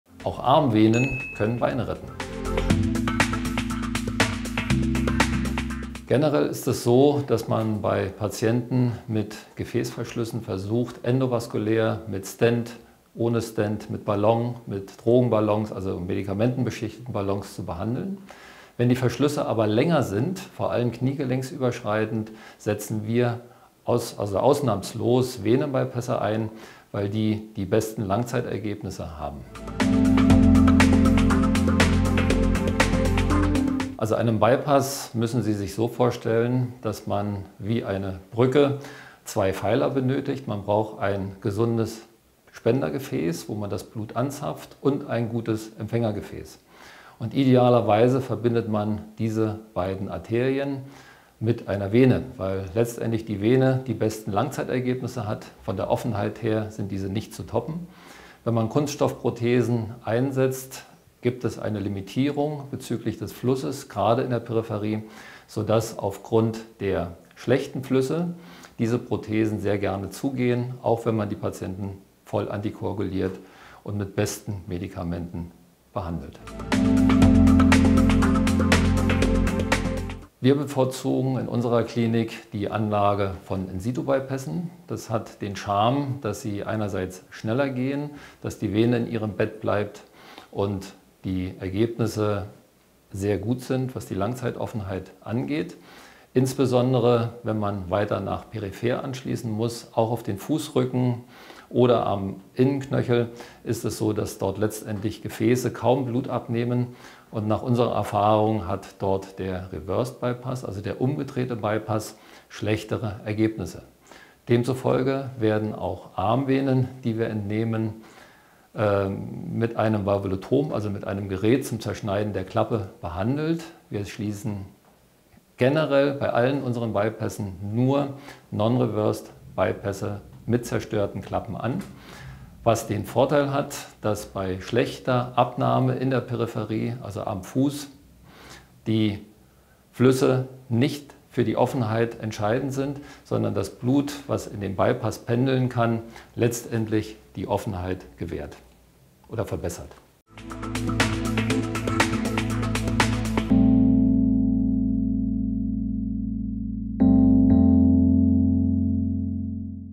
PAVK: Wann ist ein Bypass nötig und wie hilft ein Venenbypass?: 3 Fragen 3 Antworten | Asklepios
Ein Venenbypass wird bei peripherer arterieller Verschlusskrankheit (PAVK) eingesetzt, wenn andere Behandlungen nicht ausreichen. Dabei wird ein Abschnitt einer Vene, meist aus Bein oder Arm, verwendet, um das Blut um blockierte Arterien herumzuleiten. Dies kann Schmerzen lindern, die Beweglichkeit verbessern und sogar Amputationen verhindern. Dr. Mario Kuhnert, Chefarzt für Gefäß- und endovaskuläre Chirurgie an der Asklepios Klinik Birkenwerder erklärt, wann ein Bypass bei PAVK nötig ist, welche Herausforderungen es dabei gibt und wie Venen als Bypässe verwendet werden.